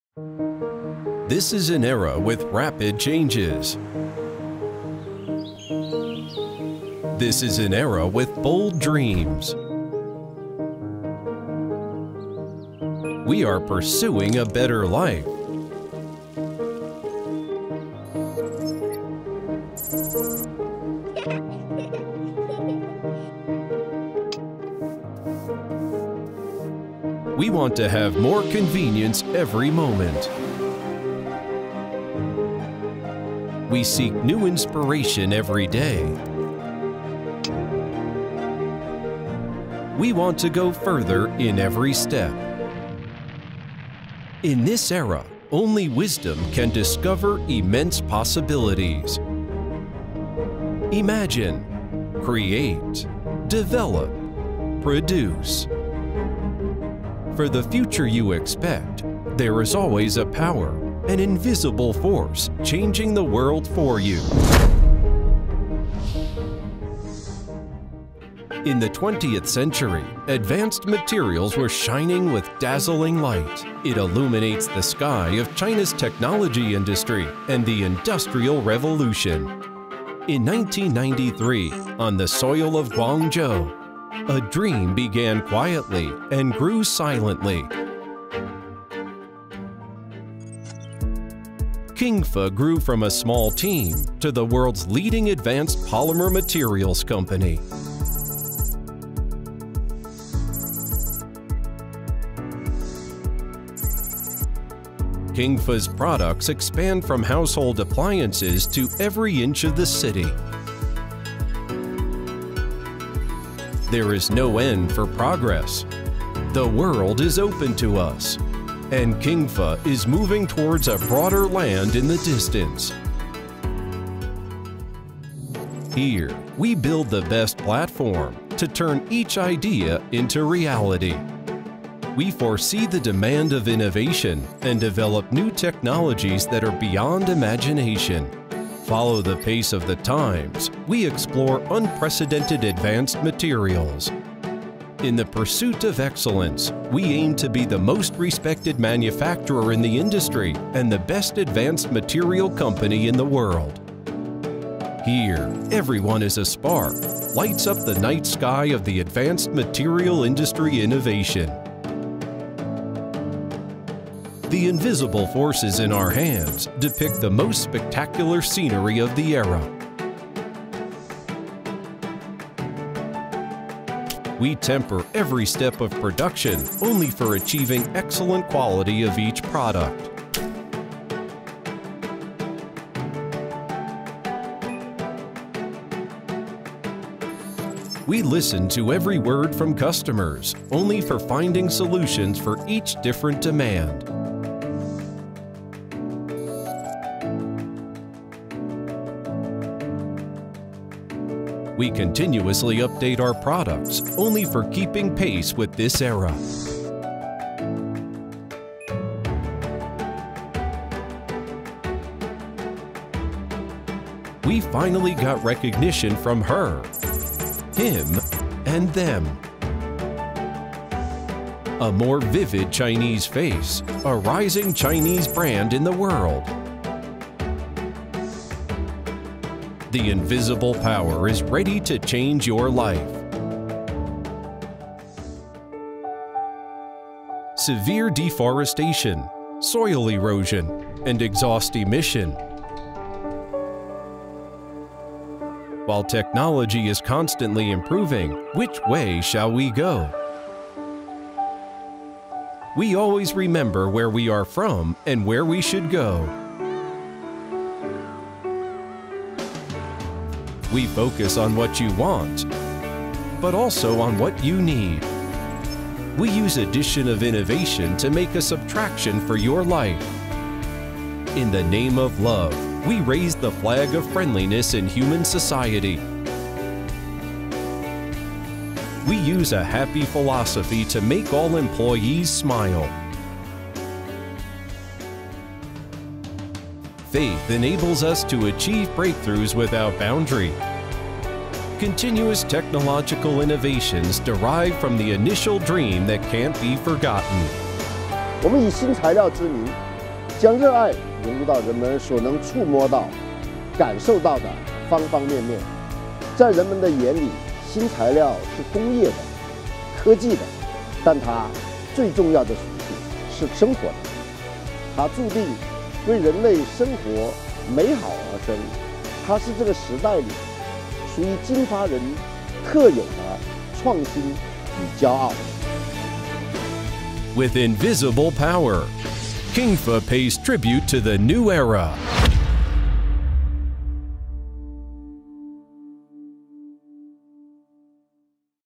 Kingfa Science & Technology Limited, a globally supplier of high-performance composite materials
Kingfa is a globally preeminent supplier of advanced materials. Based on research, manufacture, sales and services of innovative materials, Kingfa provides new solutions to create a safer, more comfortable and convenient life.

Headquartered in Guangzhou Science Town, Kingfa owns 47 subsidiaries, our R&D and production bases are established in overseas regions including South Asia, North America and Europe. The products of Kingfa mainly rely on independent innovation and development, covering six categories with independent intellectual property rights including modified plastics, fully biodegradable plastics, high-performance carbon fiber and composite materials, specialty engineering plastics, light hydrocarbons and hydrogen energy, and eco-friendly high-performance recycled plastics. With its good environmental friendliness and excellent performance, Kingfa’s materials are exported to over 130 countries and regions, serving more than 1,000 well-known companies around the world.

Kingfa is committed to solve the increasingly serious environmental problems our planet is facing, and we are following the principles of sustainable development, eager to find a balance between economic, environmental and social sustainability.